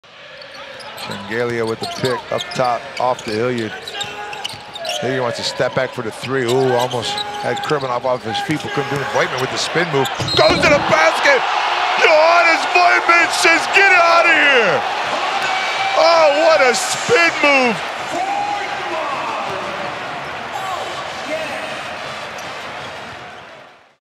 7DAYS Magic Moment of the Night: Johannes Voigtmann, KIROLBET Baskonia Vitoria-Gasteiz
Johannes Voigtmann throws down the monster slam